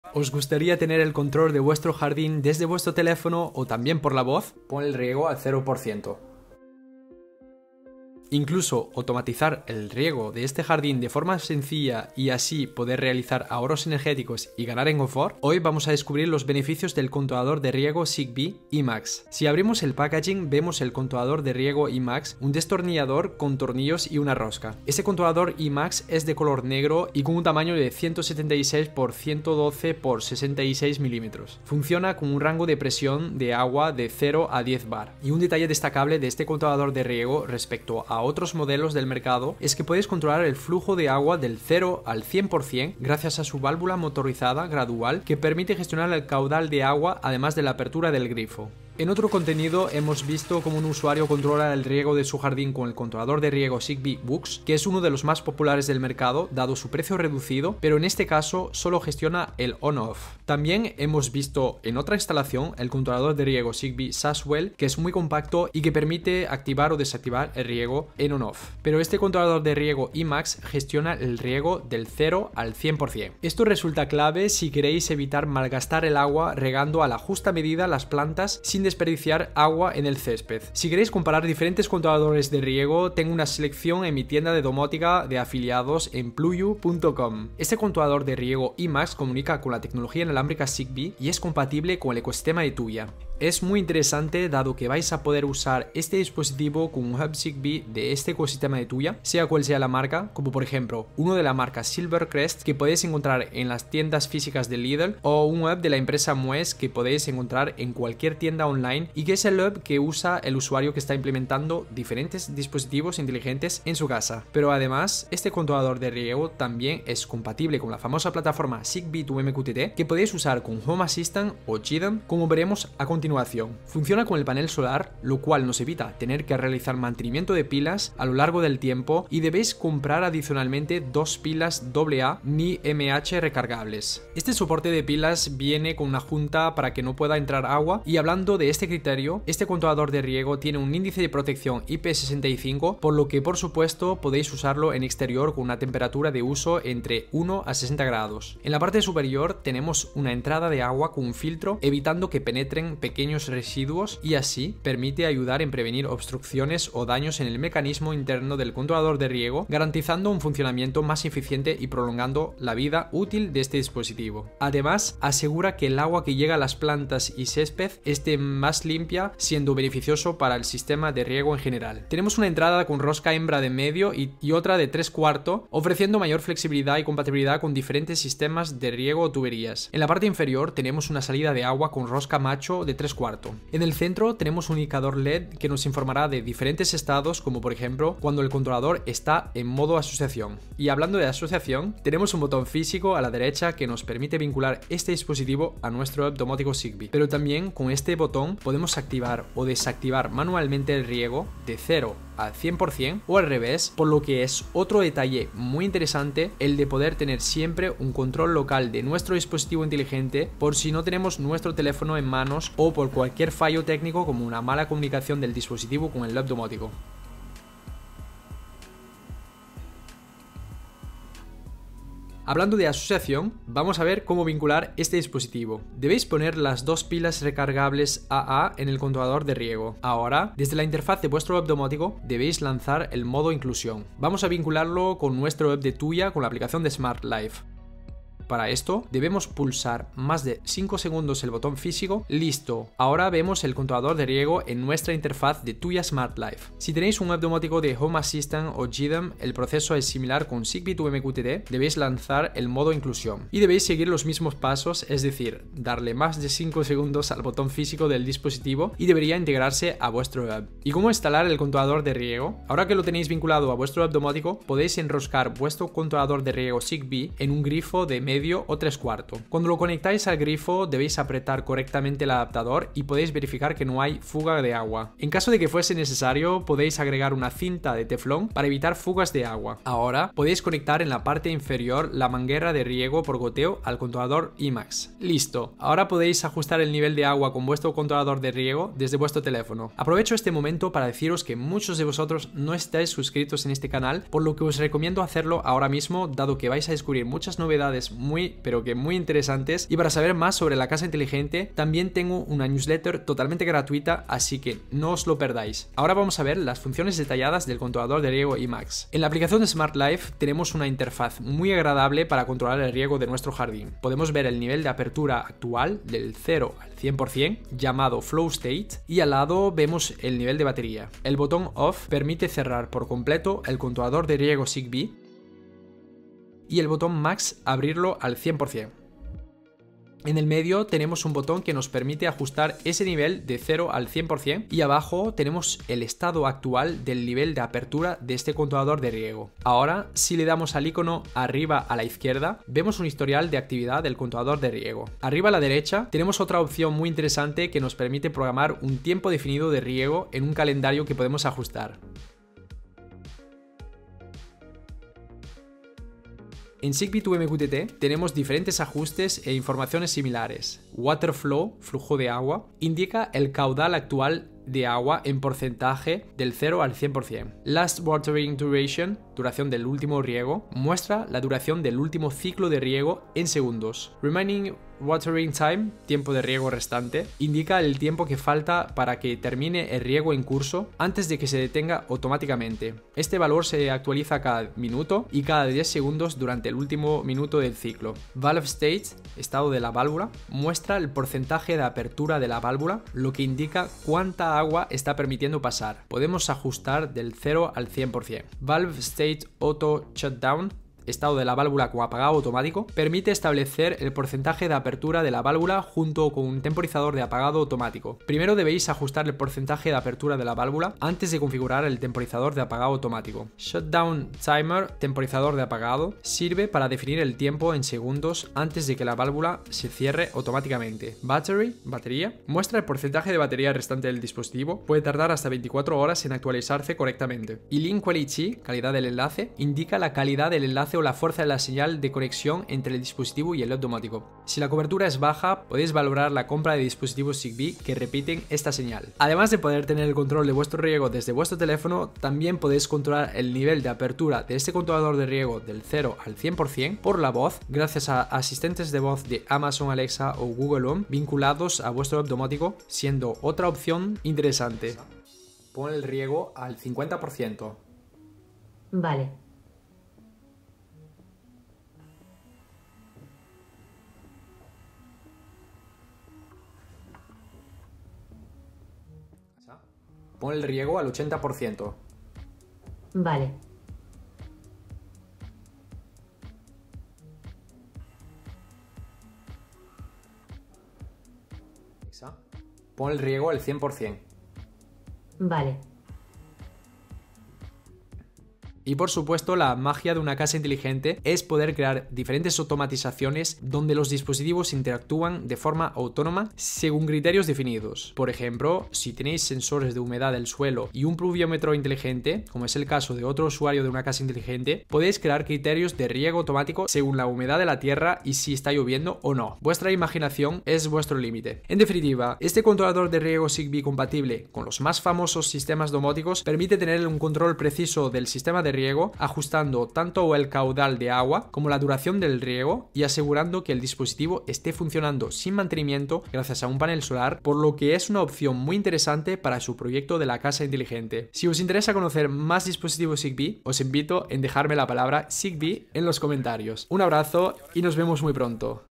QOTO QT-05M: Riego Zigbee ajustable del 0 al 100% y con Panel Solar !
Immax 07524L/QOTO QT-05M es un controlador de riego inteligente compatible Tuya Smart Life y Zigbee2MQTT (Home Assistant y Jeedom).

¿Quedamos en Contacto?